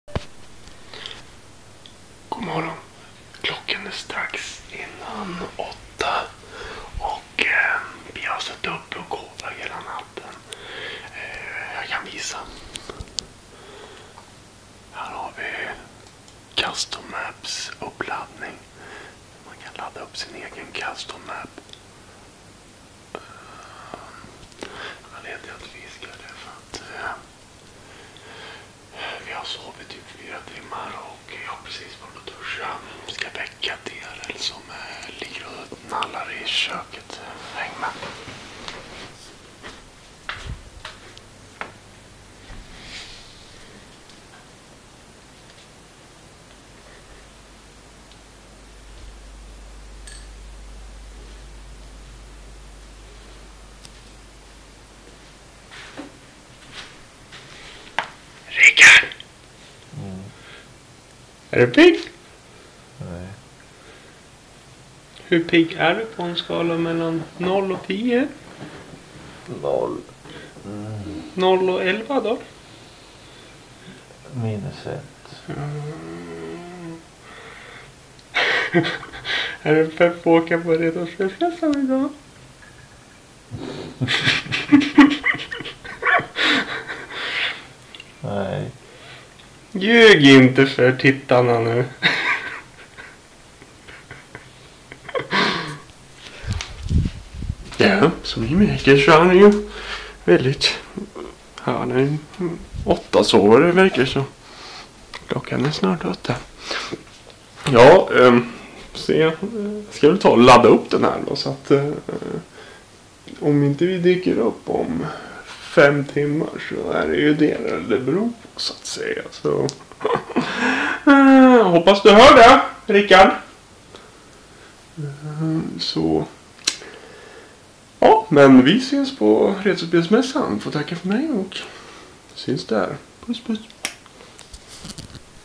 Slogg - Morgonen inför Retrospelsmässan.
En S(pontanv)logg såhär på morgonen inför retrospelsmässan.